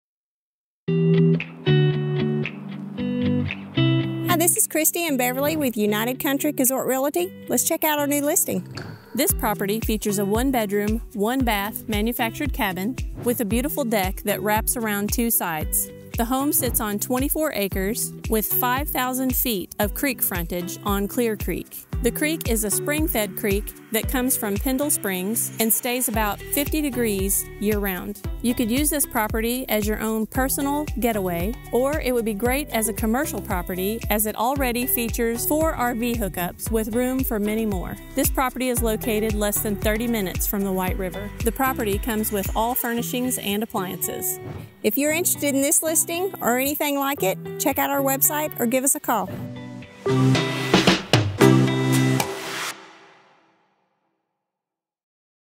Home and Acreage with LIVE WATER | 2712 MC 4021
Don't miss this amazing property located on the banks of Clear Creek! Property features 5,000 feet of year round creek frontage with breathtaking views of the beautiful bluffs and waterfalls. Property has a great swimming hole and good fishing! Home includes 1 bedroom, 1 bath, nice open concept for living, dining and kitchen, 600 sq ft of covered deck to help you relax and enjoy your perfect view! Home is furnished and ready for you to set up your vacation spot, AirBNB, or forever home! Property includes private well, septic, 2 sheds, 4 RV hookup's with room for many more! Could be a great income opportunity! Be sure to call and set up your private showing as this property will not last long!